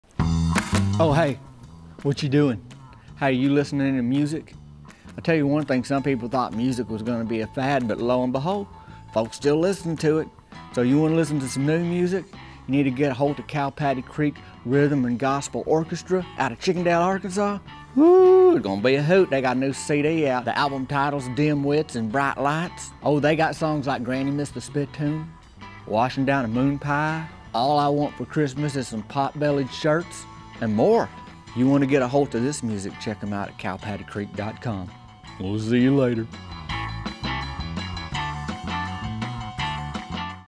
Dim Wits in Bright Lights promo by Tavin Dillard!
we got Tavin to plug our new CD!